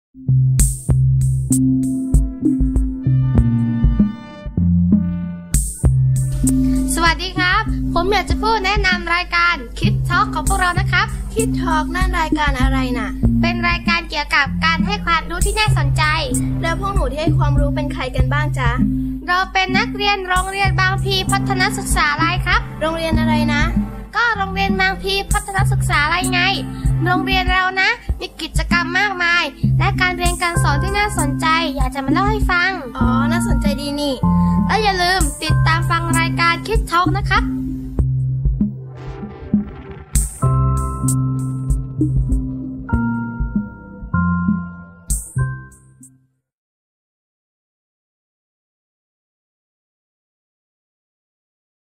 Kid Talk Spot 04- Our School
Spot for a radio program recorded entirely by students at Bangpleepattanasuksalai School.